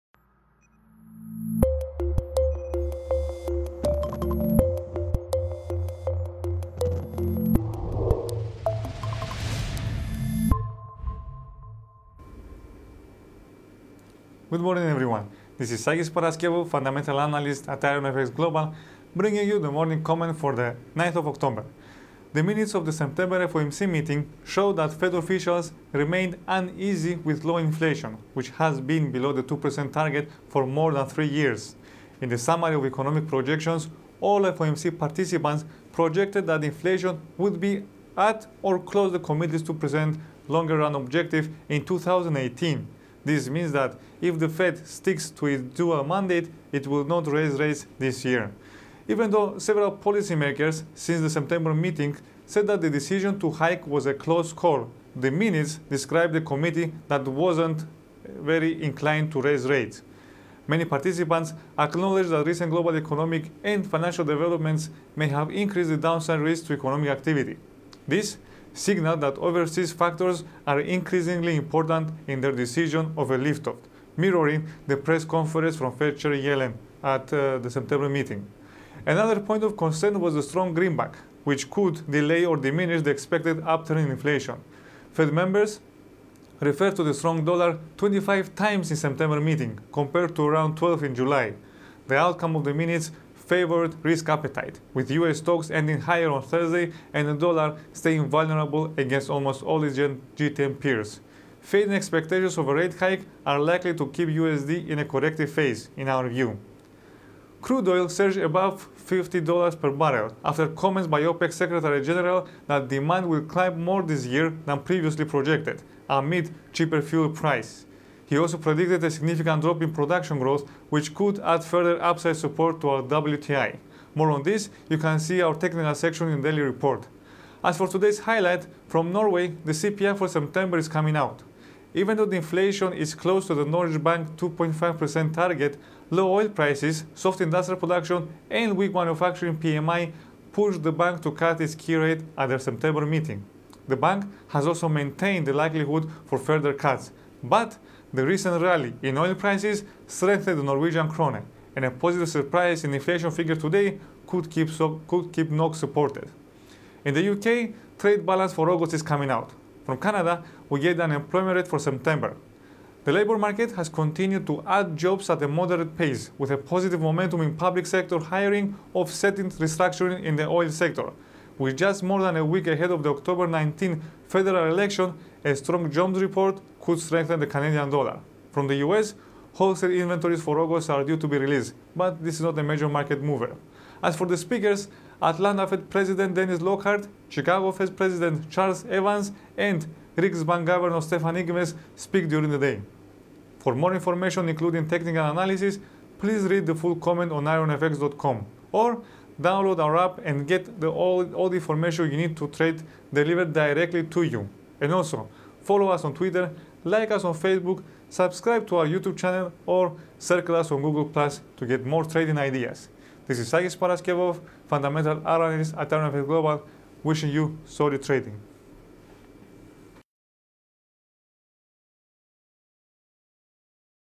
IronFX Daily Commentary | 09/10/2015
Main points for Today

• Fed minutes: September decision was not a close call
• The minutes showed that Fed officials remained uneasy with low inflation, which has been below the 2% target for more than three years.
• Even though several members said recently that the decision to hike was a close call, the minutes described a committee that wasn’t very inclined to raise rates.
• Recent global economic and financial developments have increased the downside risks to economic activity. This signalled that overseas factors are increasingly important in their decision of a lift-off, mirroring the press conference from Fed Chair Yellen.
• Another point of concern was the strong greenback, which could delay or diminish the expected upturn in inflation.
• Fading expectations of a rate hike are likely to support riskier assets.
• Today:
• France: Industrial production for August. Usually not major market mover.
• Norway: The CPI for September is forecast to rise, this could be NOK-positive at least temporarily.
• UK: Trade balance for August.
• Canada: Unemployment rate for September. It is expected to remain unchanged at 7.0%, while net change in employment is expected to show another increase. This could strengthen CAD.
• US: Wholesale inventories for August.
• Speakers: Atlanta Fed President Dennis Lockhart, Chicago Fed President Charles Evans, Riksbank Governor Stefan Ingves speak